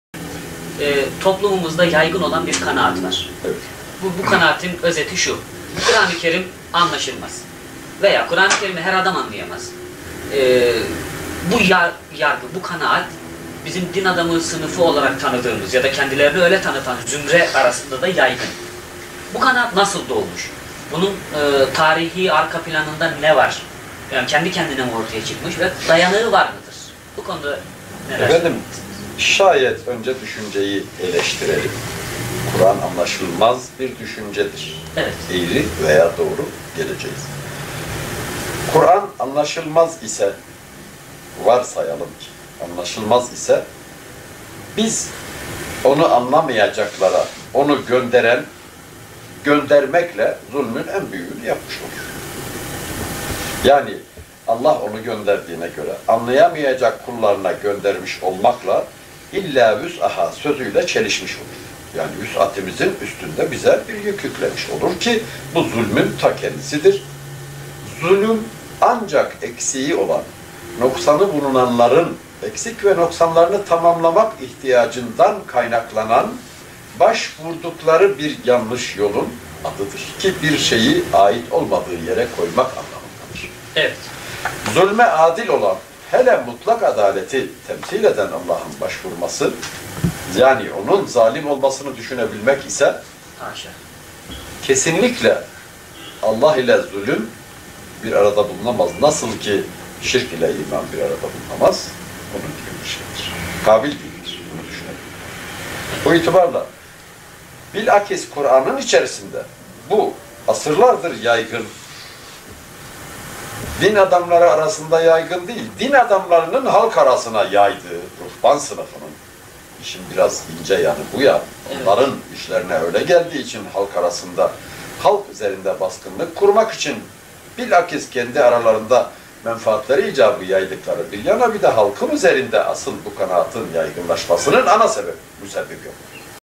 Kuran anlaşılmaz iftirasının kaynağı din adamlarıdır - Ercümend Özkan
Kuran anlaşılmaz iftirasının asıl kaynağı bizzat onu halka öğretmesi gereken din adamlarıdır. Kuran'ın halk tarafından anlaşılması çoğu din adamının işine gelmez çünkü Kuran'ı anlayan kişiler geleneksel hurafeleri reddeder. Ayrıca din adamlarına olan ihtiyaç azalır. Din ne kadar karışık olursa din adamı da o kadar değerli olur.